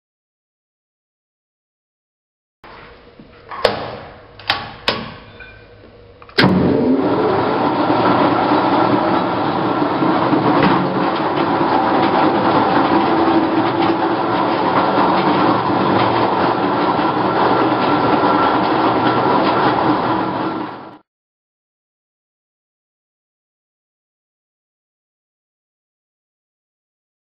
Prasa mimośrodowa LEN 40 C
Prezentacja pracy prasy mimośrodowej LEN 40 C.

Dane techniczne:
- nacisk nominalny: 40 Ton,
- liczba skoków suwaka przy ruchu ciągłym: 70 lub 142 /min,
- liczba skoków pojedynczych: 50/min,
- regulowany skok suwaka: 8-95 mm,
- wielkość regulacji miedzy stołem, a suwakiem: 80 mm,
- wysięg: 265 mm,
- odległość między stołem a suwakiem: 295 mm,
- powierzchnia stołu: 660 x 530 mm,
- powierzchnia suwaka: 400 x 250 mm,
- wymiary gabarytowe (długość x szerokość x wysokość): 1585 x 1245 x 2370 mm,
- sprzęgło pneumatyczne (min. ciśnienie sprężonego powietrza: 0,4 MPa,)
- moc silnika: 3/4 kW,
- masa: 4500 kg.